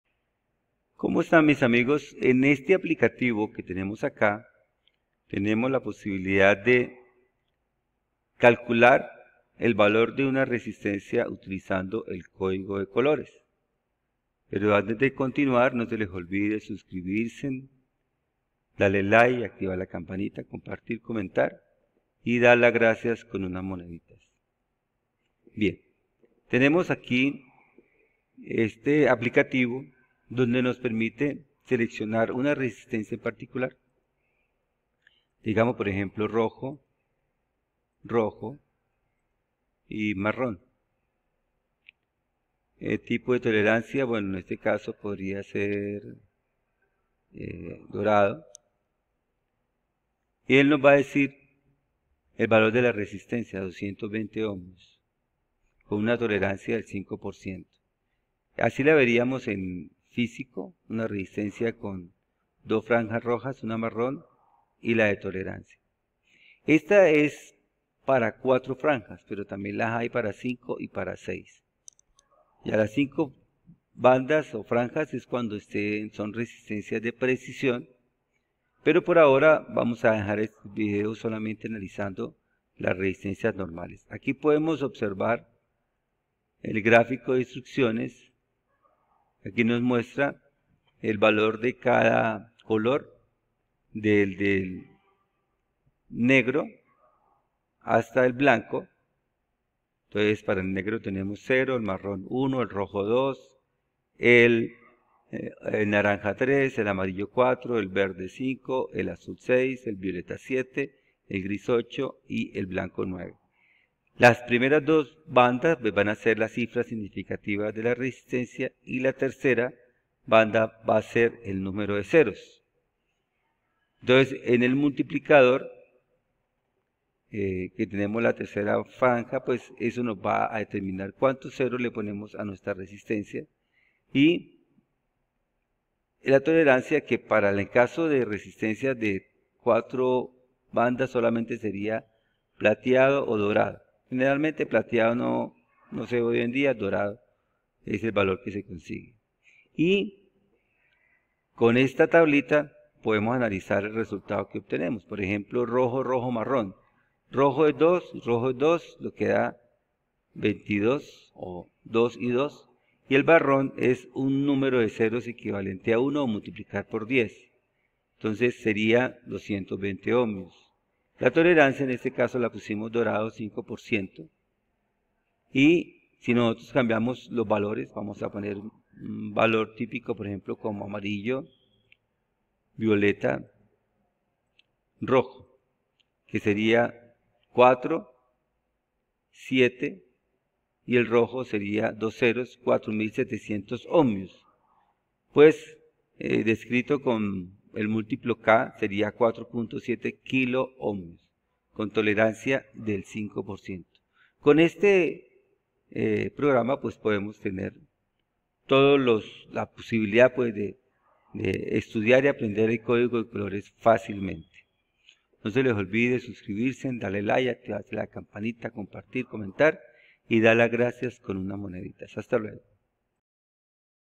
aplicativo para encontrar el valor de una resistencia con el còdigo de colores
en este video podemos ver un aplicativo para calcular el valor de una resistencia con el còdigo de colores